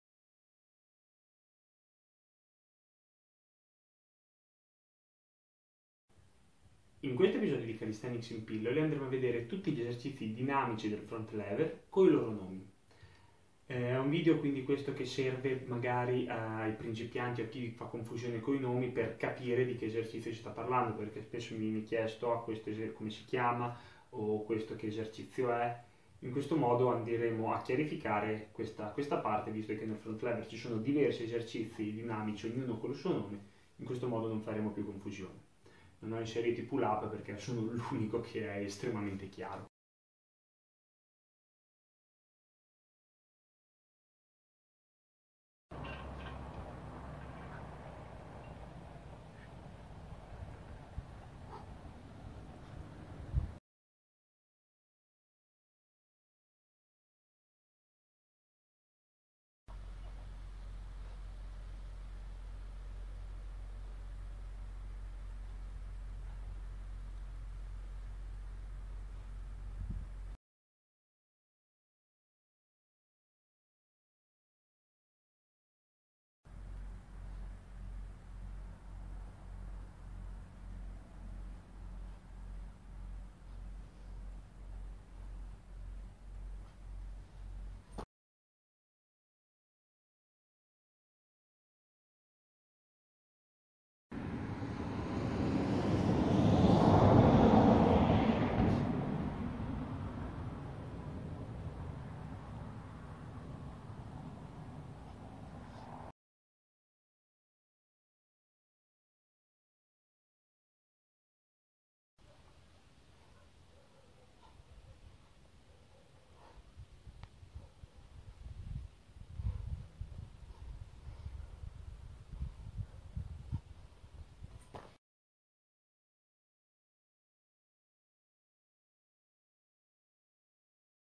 Calisthenics in pillole ep. 46: Nomenclatura Front Lever
In questo episodio vedremo i nome degli esercizi dinamici del front lever.